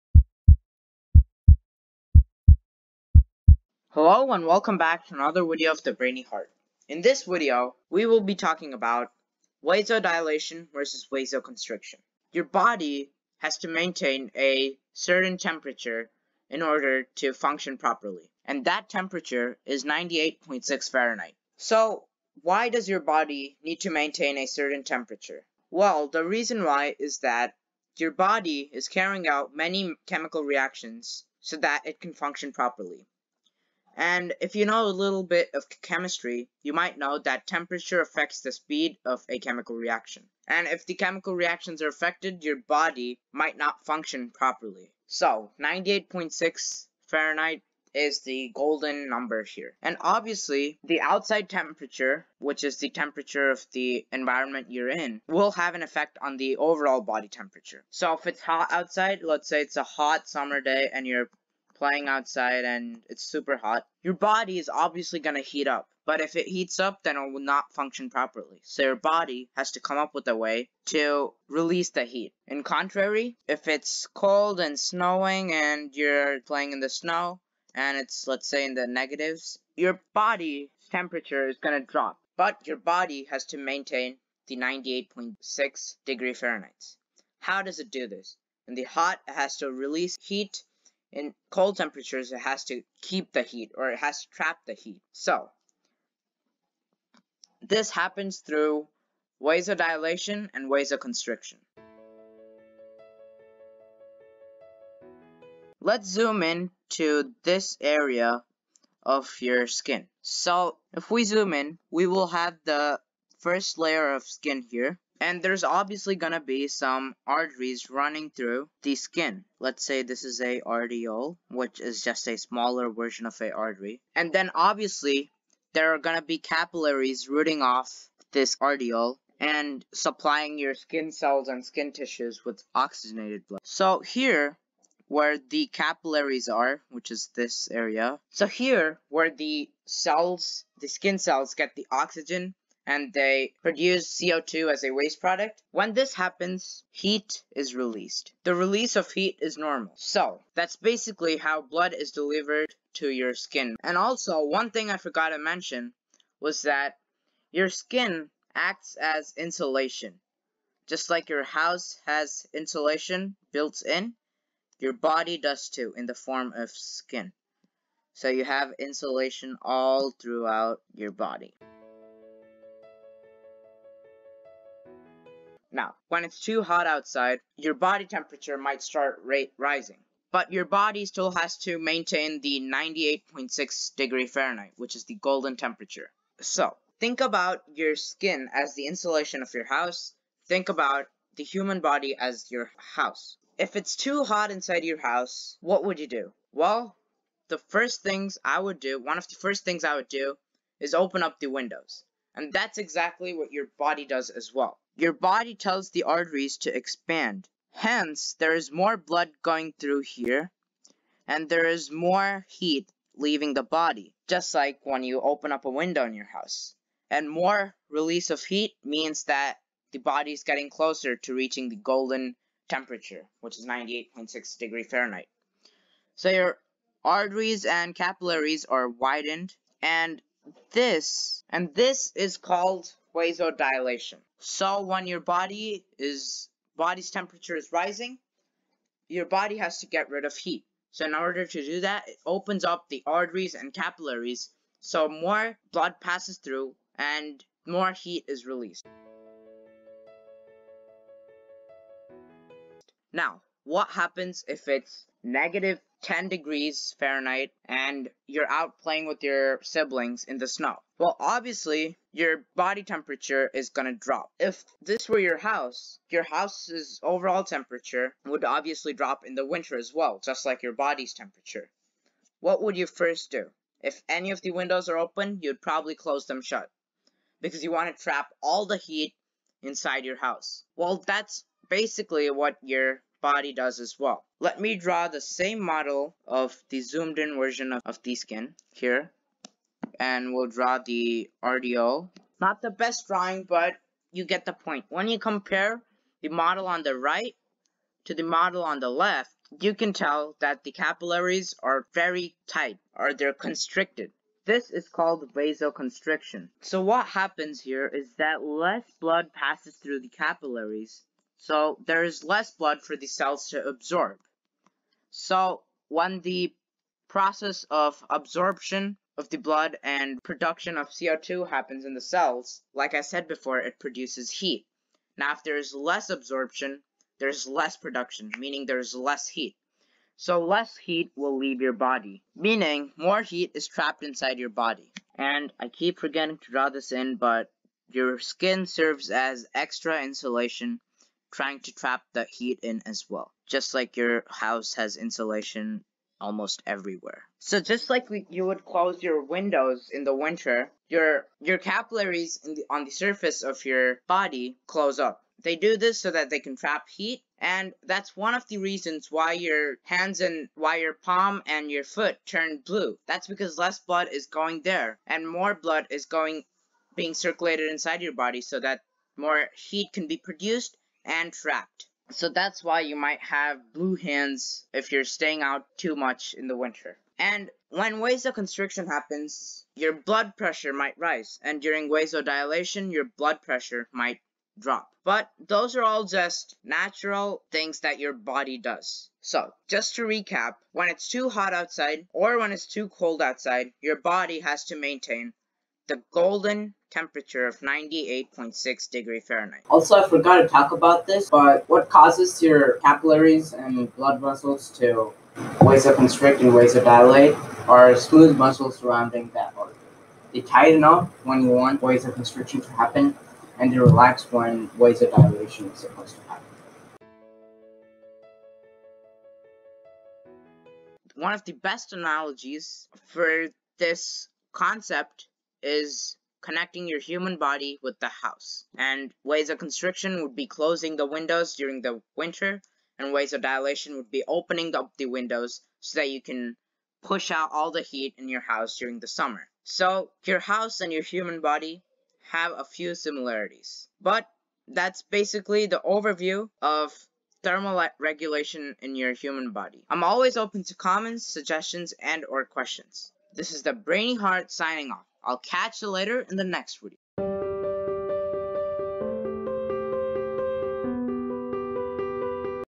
Intro to Vasoconstriction & Vasodilation
In today's video, you will learn about a type of thermoregulation, the ability of an organism to keep its body temperature within certain boundaries, even when the surrounding temperature is very different, that has to do with your circulatory system and more specifically your blood vessels. In today's video, I talked about vasodilation and vasoconstriction. I also gave a very good analogy so that it is easier for you to understand and memorize the anatomy & physiology behind the concept. As always, hope you learn a little and enjoy during the process. I will catch you later in the next one!

Time Stamps:
Intro: (0:00)
Section 2: Skin & Blood: (1:41)
Section 3: Vasodilation: (2:51)
Section 4: Vasocontriction: (4:08)
Section 5: Analogies & Overview: (6:56)
Outro: (7:25)